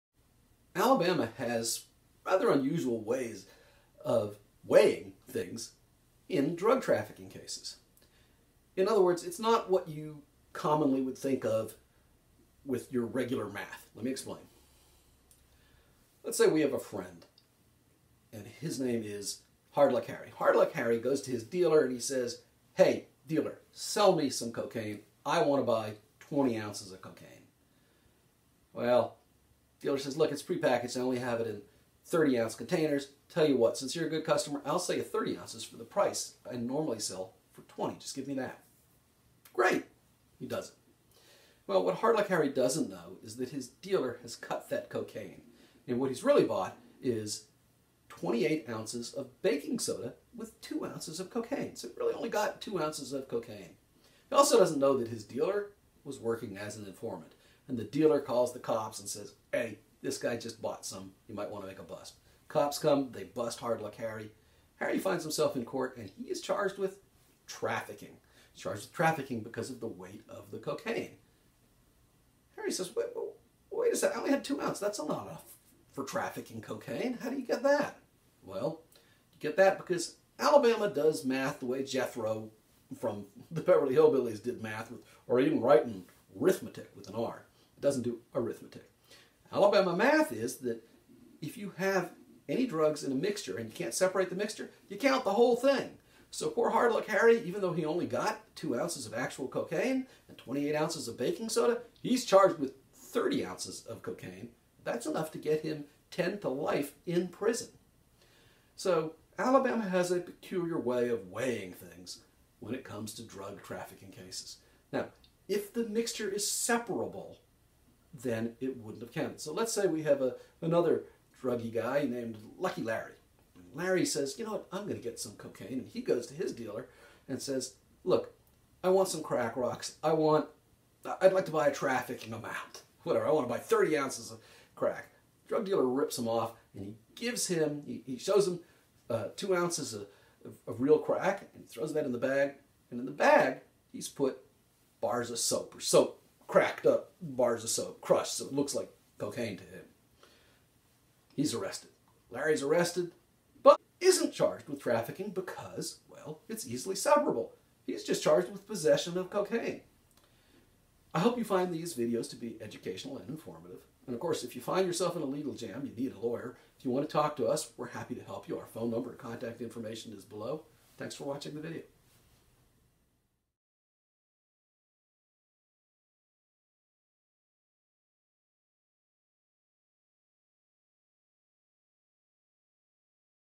How Alabama weighs drugs in Drug Trafficking or Strange Weights and Measures
Alabama has rather unusual ways of weighing things in drug trafficking cases. In other words, it's not what you would commonly think of with your regular math. Let me explain. Let's say we have a friend and his name is "Hard Luck Harry.". He goes to his dealer and says "hey, dealer sell me some cocaine, I want to buy 20 g."

Well, the dealer says "well  I only have 30-gram packages, tell you what, since your good customer, I will sell you 30 ounces for the price 0f 20.

Hard Luck Harry says "Great".

He buys the cocaine.

Harry doesn't know his dealer has cut that cocaine and he's really bought  28 grams of baking soda with 2 grams of cocaine. Harry also does know that his dealer has been working as an informant.

The dealer calls the cops and says Harry just got some drugs. The cops bust  Harry. Harry finds himself in court charged with Trafficking in Cocaine.

He is charged with trafficking because of the weight of cocaine. See section 13 A – 12 – 231 of the Alabama Code.

Harry says "Wait a minute, I only had 2 grams,  that's not fair".

So how do you get 30 grams? You get that because Alabama Does Math the way Jethro, from the Beverly hillbillies, did math: "Readin, Rightin, Rithmatic!"

Alabama math is that if you have any drugs in a mixture and you can't separate the mixture you count the whole thing.

So for Hard Luck Harry, even though he only got 2 grams of actual cocaine, and, 20 grams of baking soda; he is charged with all 30 grams. That's enough to get Harry a trafficking charge. If he's convicted of trafficking he's looking at in life in prison.

So Alabama has a peculiar way of weighing things. When it comes to drug trafficking cases, if the mixture is separable then it doesn't count.

Say we have another druggy guy named Lucky Larry. Lucky Larry decides to get cocaine. He goes to his dealer and says "I would like to buy a trafficking amount of crack rocks, 30 grams ". The drug dealer sells Lucky Larry 2 grams of real cocaine and crushes up some ivory soap so it looks like rocks of cocaine. He sells Lucky Larry 30 grams of this.

Larry is arrested. Larry is not charged with trafficking. That's because this mixture of crack and "soap-rocks" is easily separable. Larry is only charged with possession of cocaine because it is easy to separate the actual rocks of cocaine from the fake rocks of cocaine that are soap.

I hope you find these videos to be educational and informative.